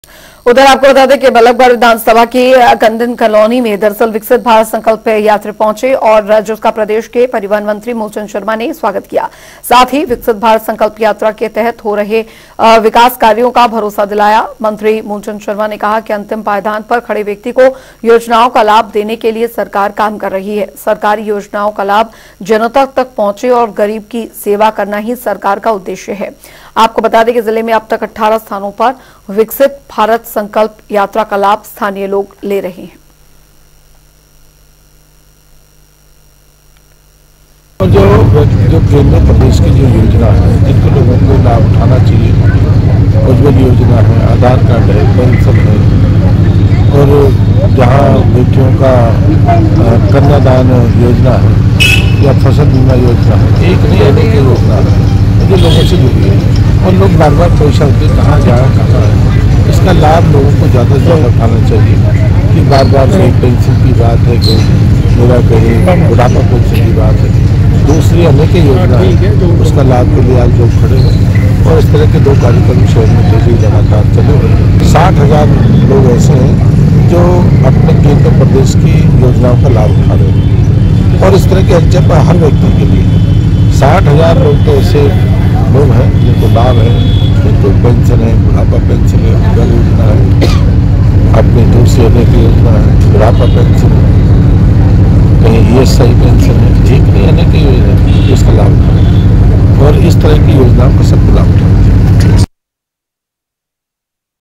Viksit Bharat Sankalp Yatra का Moolchand Sharma ने किया स्वागत, बोले- गरीब की सेवा करना सरकार का...
Viksit Bharat Sankalp Yatra का Moolchand Sharma ने किया स्वागत, बोले- गरीब की सेवा करना सरकार का उद्देश्य